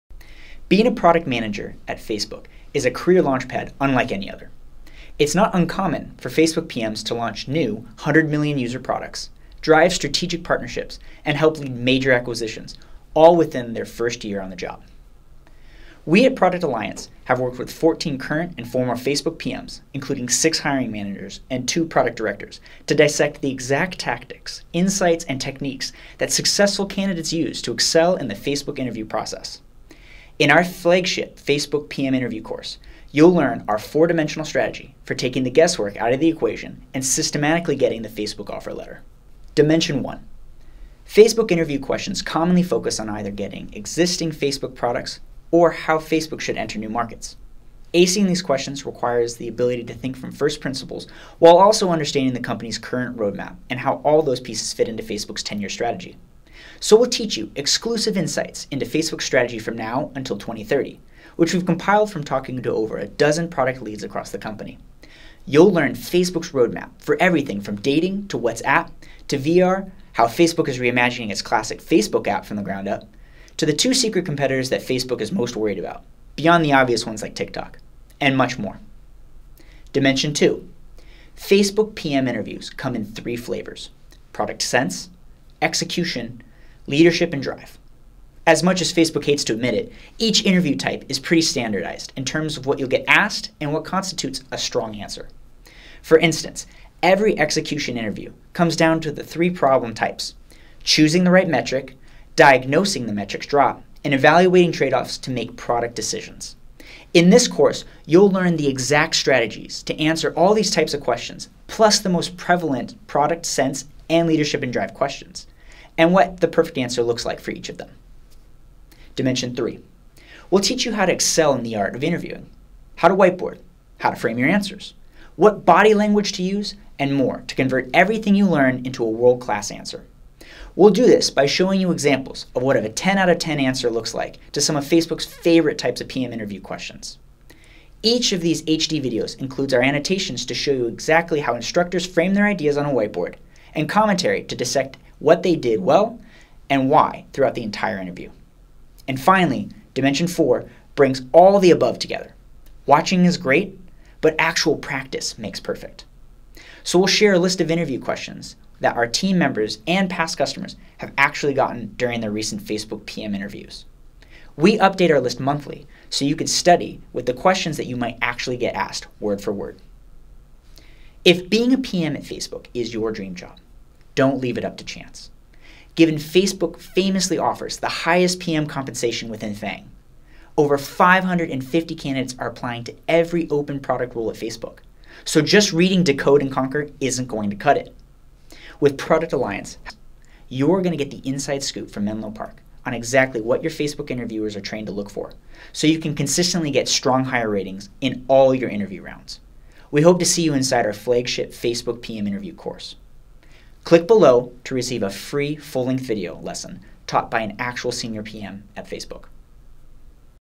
The Flagship Meta PM Interview Course | Product Alliance
Interviewing for a PM role at Meta? We worked with 14 current and former Meta PMs to create the Flagship Meta PM Interview course. Learn our four-dimensional strategy for systematically getting the Meta PM offer letter

In our course, you’ll start off by learning Meta’s strategy between now and 2030 for the core app, Instagram, Whatsapp, and more. Given how often Meta product interview questions focus on either existing Meta products or how Meta should enter new markets, understanding the C-Suite’s long term vision is absolutely critical for getting strong hire ratings.

From there, we will give you a refresher on the art of interviewing, covering everything from whiteboarding to body language. We’ll reveal every variation of Product Sense, Analytical Thinking, and Leadership and Drive questions you are likely to get asked at Meta and then walk you through the concrete things that Meta interviewers are taught to look for in your response for each question type. We’ll also show you tons of mock interview examples of 10 out of 10 answers with expert interviewer commentary along the way.

Finally, we will share a monthly updated list of interview questions that our team members and past customers have actually gotten during their recent Meta PM interviews. With this course, you can take luck out of the equation for getting your dream PM job at Meta!